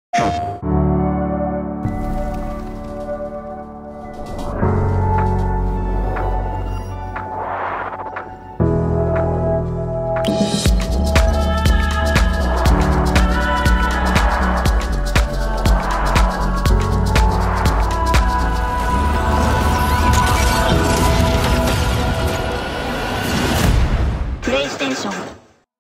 Variable Refresh Rate（可変リフレッシュレート） | PS5
PS5本体で可変リフレッシュレート（VRR）をオンにすることで、HDMI 2.1 VRR対応のテレビやPCモニターにおいて、本機能対応のPlayStation5ゲームのビジュアルパフォーマンスを向上することができます。